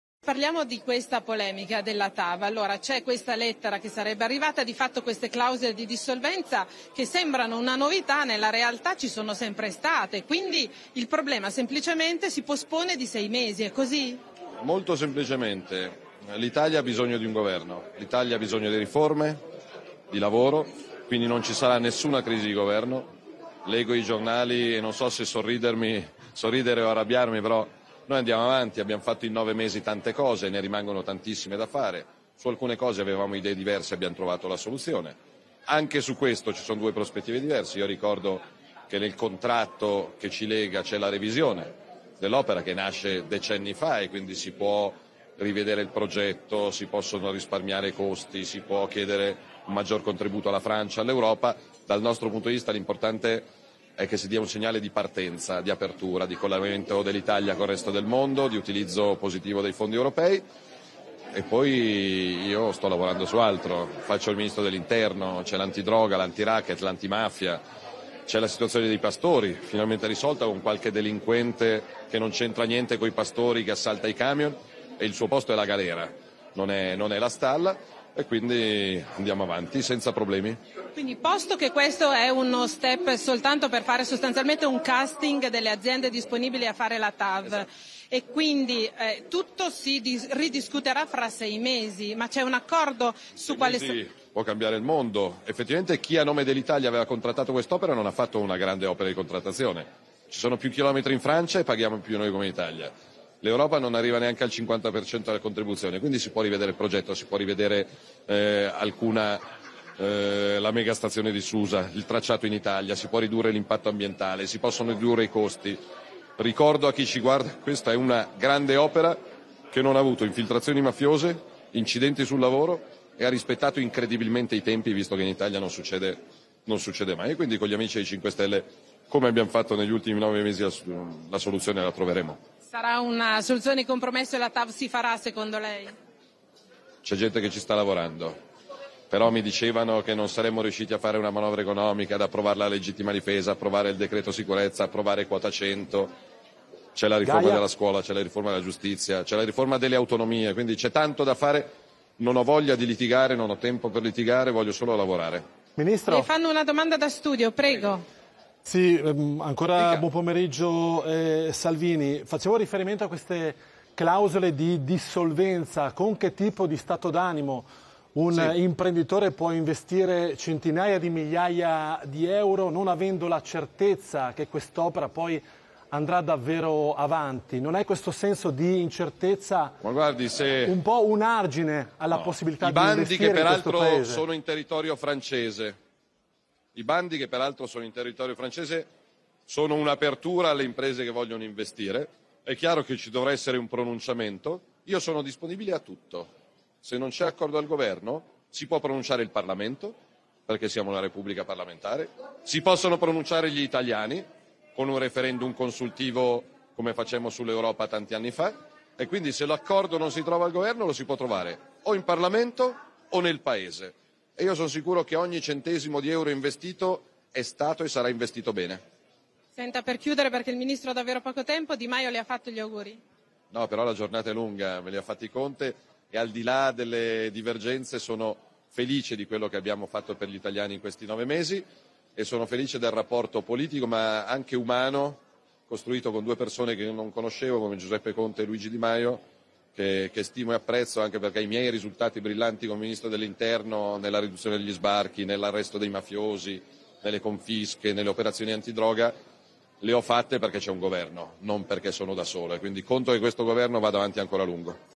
SALVINI: REGALEREMO AGLI ITALIANI 5 ANNI DI GOVERNO
La mia intervista di oggi a SkyTg24. L'Italia ha bisogno di sviluppo, di riforme, di lavoro. Altro che crisi, regaleremo agli italiani 5 anni di governo.